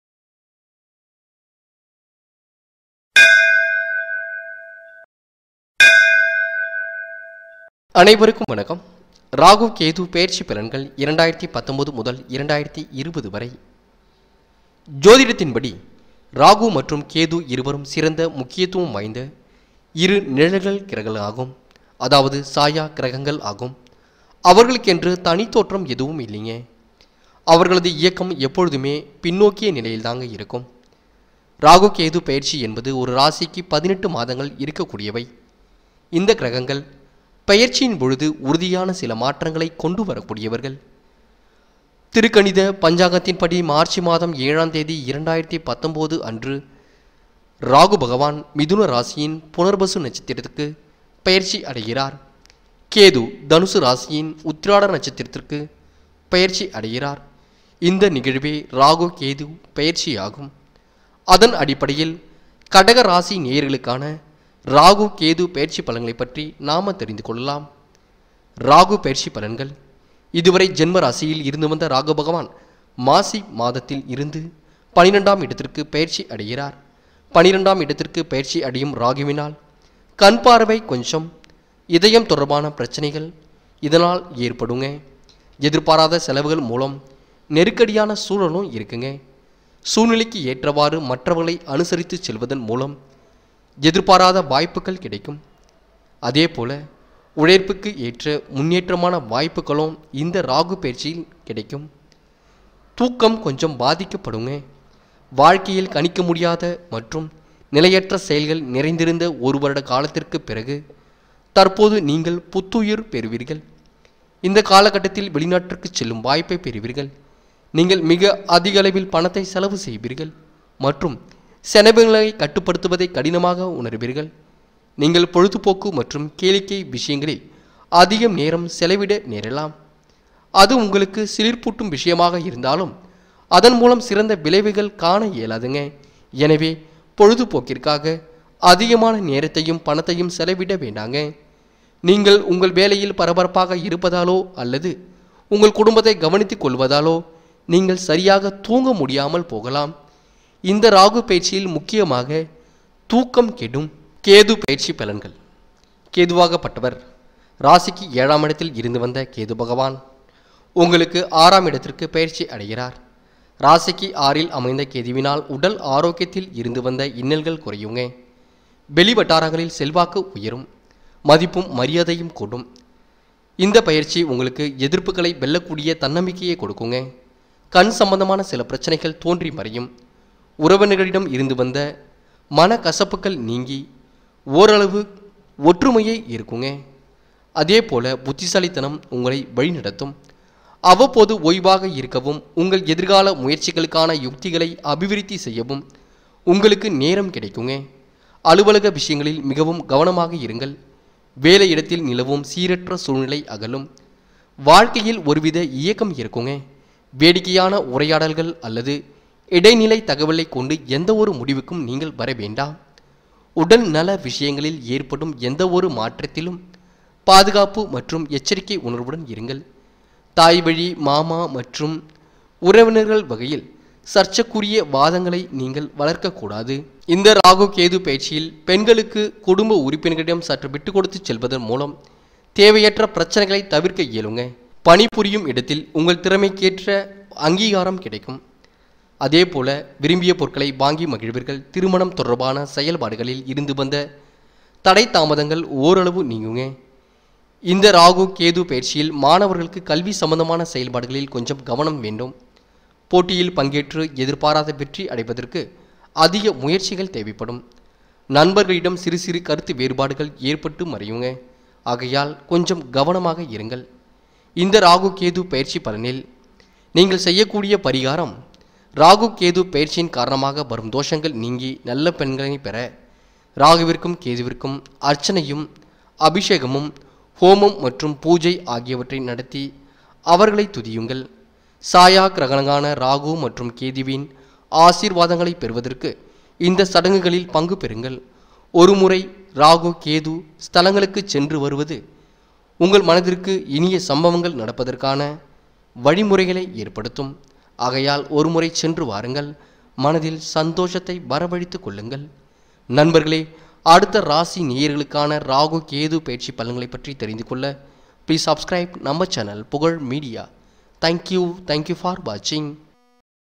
KADAGAM | இராகு-கேது பெயர்ச்சி பலன்கள் 2019-2020 | RAGHU-KETHU PEYARCHI PALANGAL
ஜோதிடத்தின்படி ராகு மற்றும் கேது இருவரும் சிறந்த முக்கியத்துவம் வாய்ந்த இரு நிழல் கிரகங்கள் / சாயா கிரகங்கள். அவர்களுக்கென்று தனித் தோற்றம் எதுவும் இல்லை. அவர்களது இயக்கம் எப்போதும் பின் நோக்கிய நிலையில் இருக்கும். ராகு கேது பெயர்ச்சி என்பது ஒரு ராசிக்கு 18 மாதங்கள் இருக்கும். இந்த கிரகங்கள், பெயர்ச்சியின் போது உறுதியான மாற்றங்களைக் கொண்டு வருவார்கள். மார்ச் 7, 2019 அன்று, ராகு மிதுன ராசியின் புனர்வசு நட்சத்திரத்திற்கு பெயர்ச்சி அடைகிறார். கேது தனுசு ராசியின் உத்திராட நட்சத்திரத்திற்கு பெயர்ச்சி அடைகிறார்.